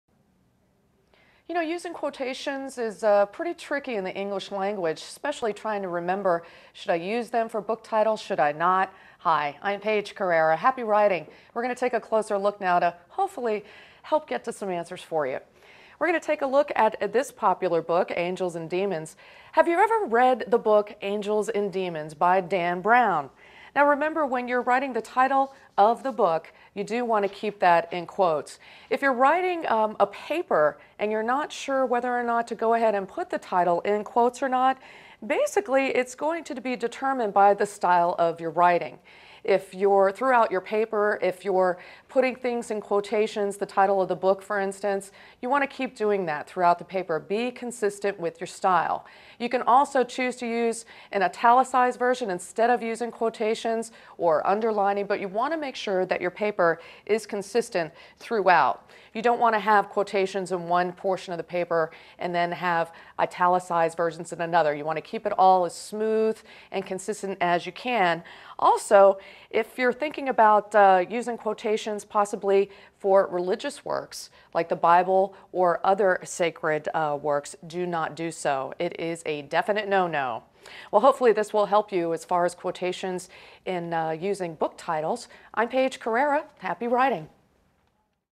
English Grammar & Punctuation : Using Quotations for Book Titles
Quotations can be used for book titles if they are used consistently, but other options include underlining and italics. Discover when it's acceptable to put book titles in quotes with help from a certified tutor in this free video on punctuation in the English language.

Expert: Paige Carrera
Bio: Paige Carrera is a certified tutor for both Hillsborough and Pinellas Counties in the Tampa Bay area of Florida.